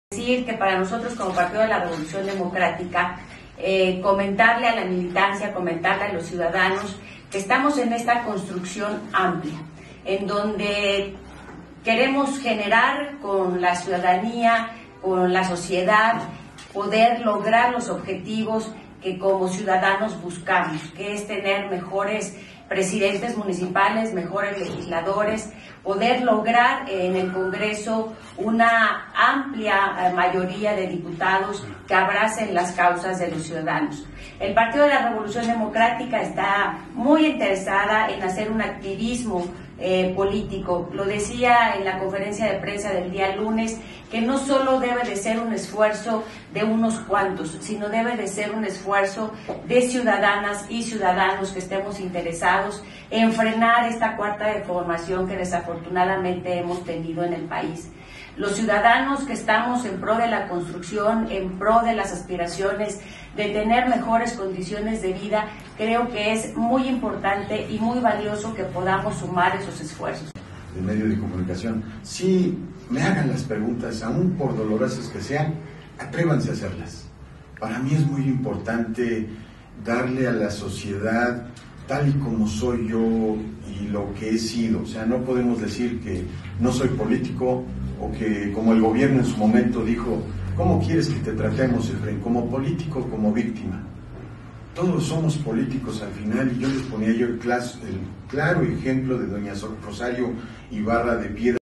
▶️ Rueda de prensa del PRD
Vía: Jaime Anzurez // EsImagen Tlaxcala

Tlaxcala, Tlax. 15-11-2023. Esta mañana en rueda de prensa, la presidenta del Partido de la Revolución Democrática (PRD) en el Estado, Patricia Zenteno, habló solo la unidad y la cohesión que esperan se dé en el frente amplio por México para los próximos comicios. Asimismo, estuvo acompañada del activista Efrén López, quién reconoció su interés por participar en las elecciones.